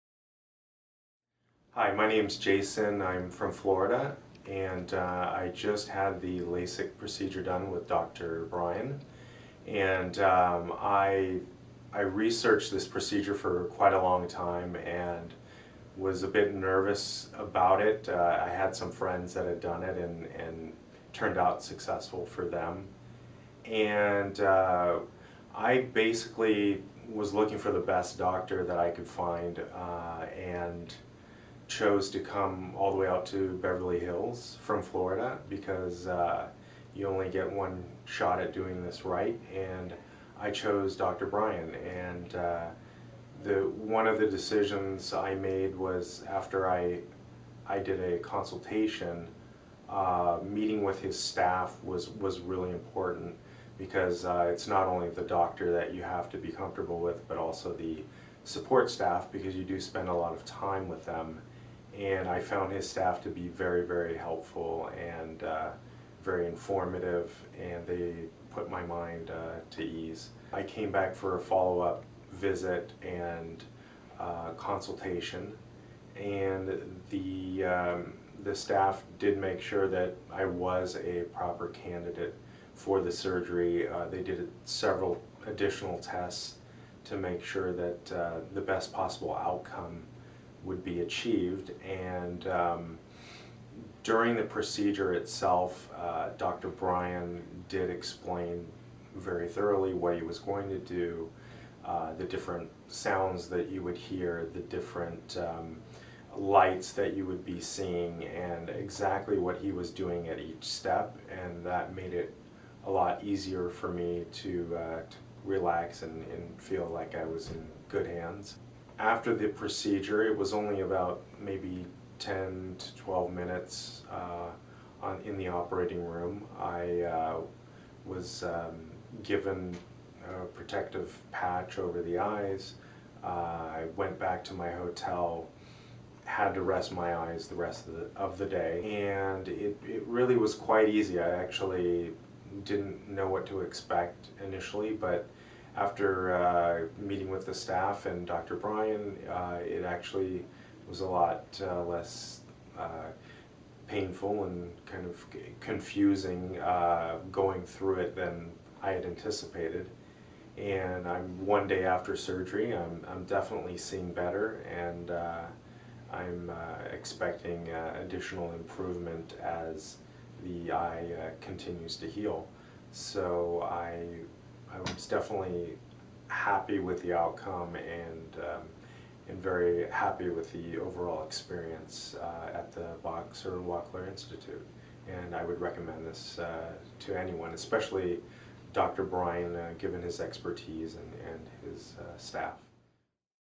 Jason from Florida had LASIK with Dr. Brian because “you only get one shot at getting it right”
Jason came from Florida was looking for the best doctor for LASIK and decided to go forward with Dr. Brian Boxer Wachler because “you only get one shot at getting it right.” Learn more about Fortified LASIK and other vision correction options with Dr. Brian Wachler